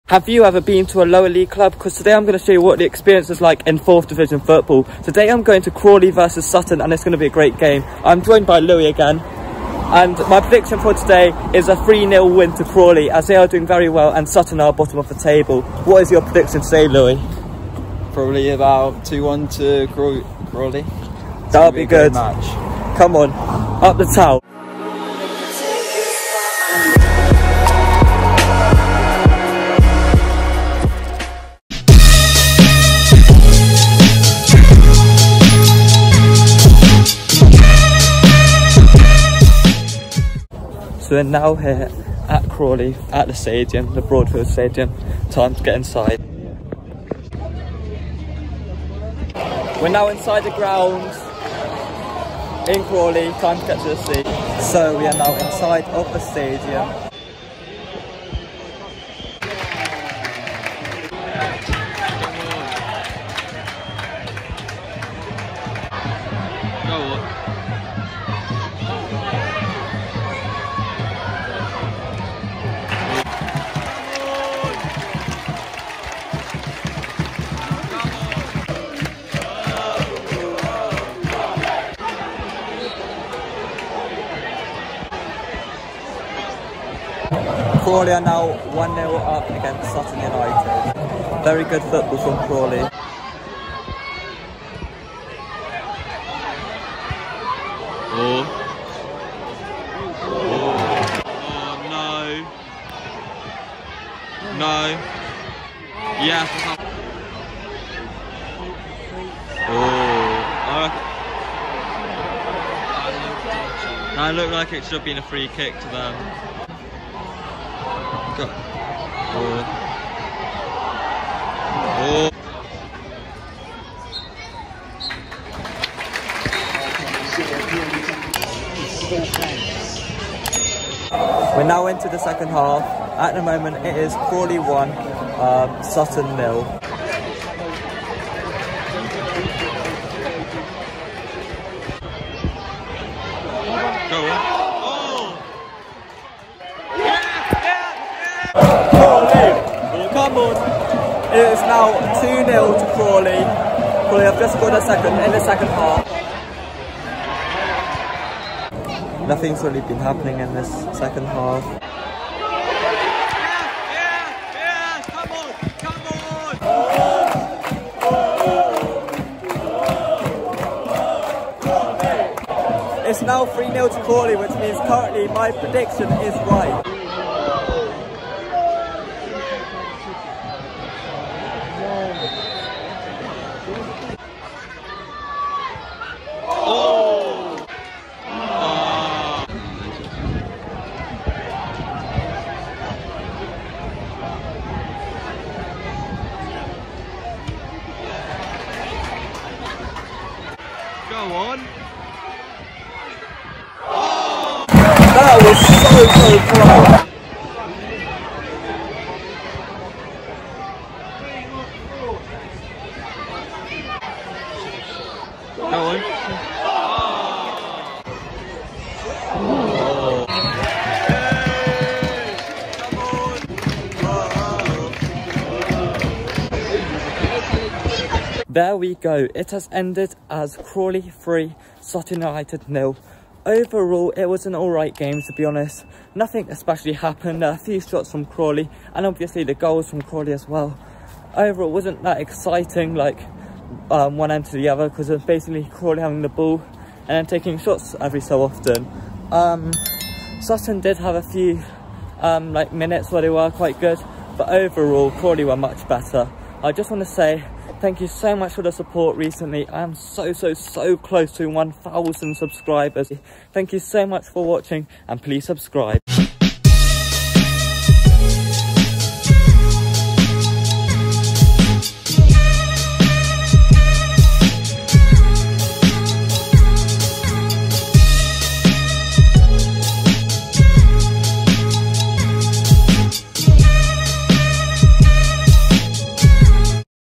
CRAWLEY 3-0 SUTTON | Matchday Vlog | Crawley are on Fire!
Crawley vs Sutton
Broadfield Stadium
Matchday Vlog
30th September 2023